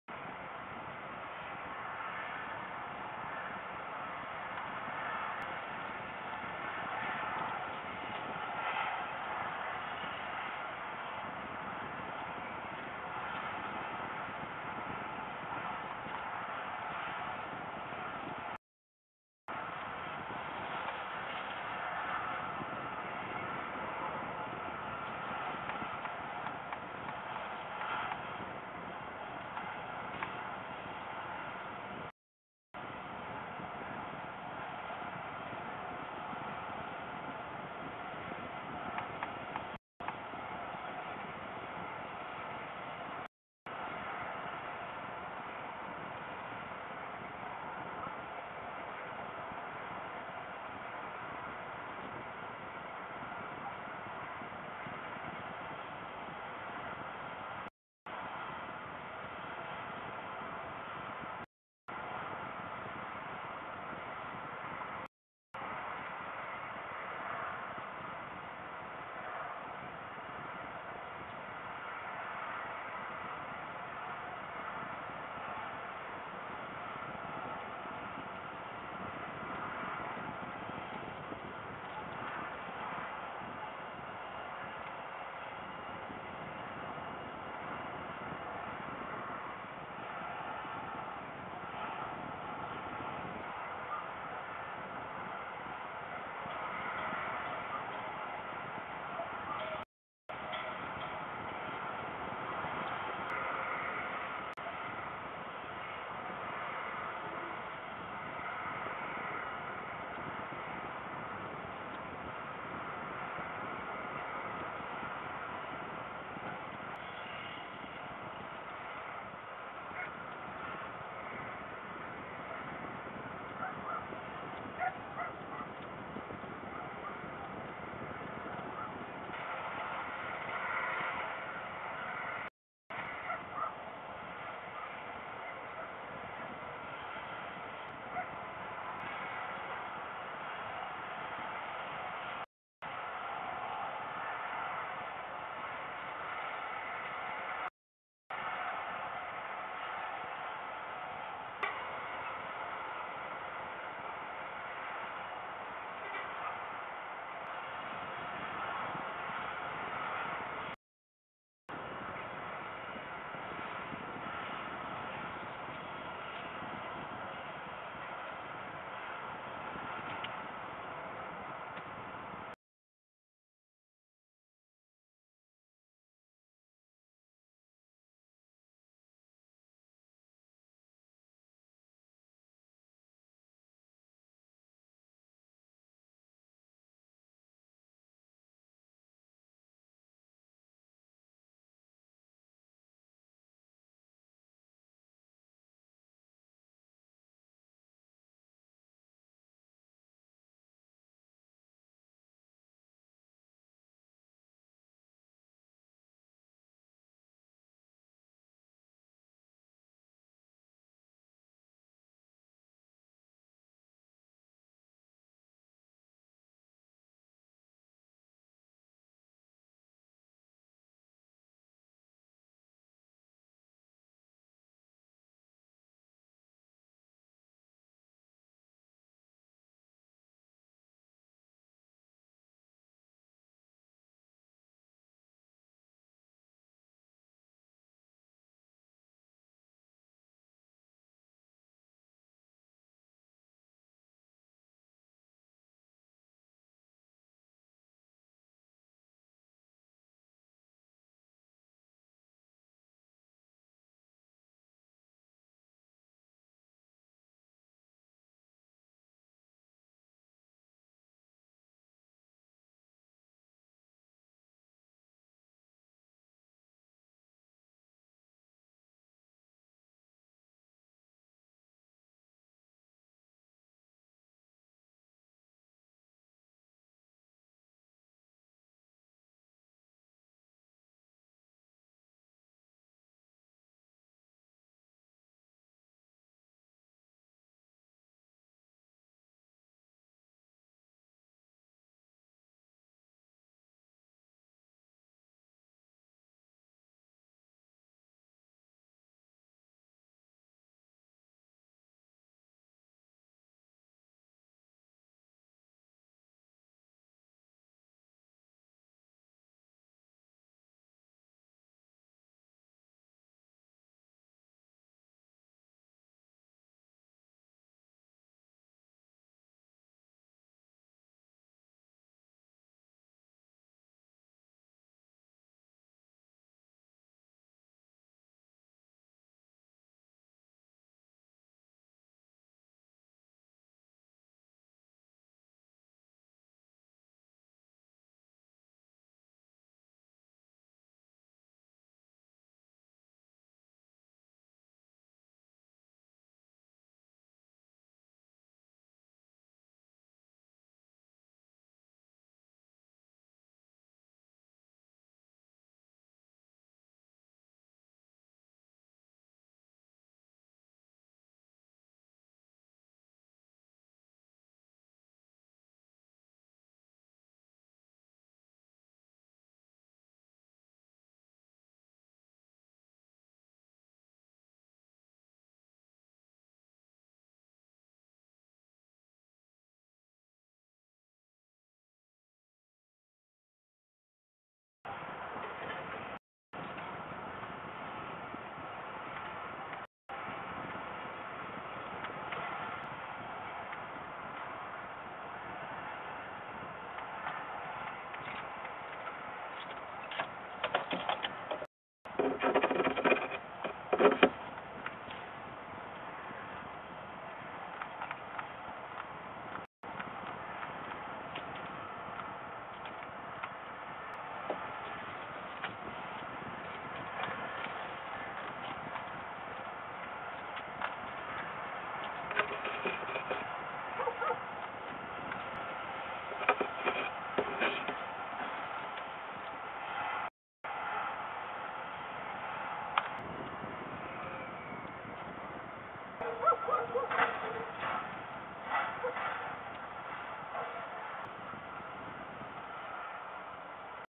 MPEC Włocławek Pl - Glinka serves her bones for lunch and goes back to the nest - 2020 01 25 11 20
but I think she'll order something to hunt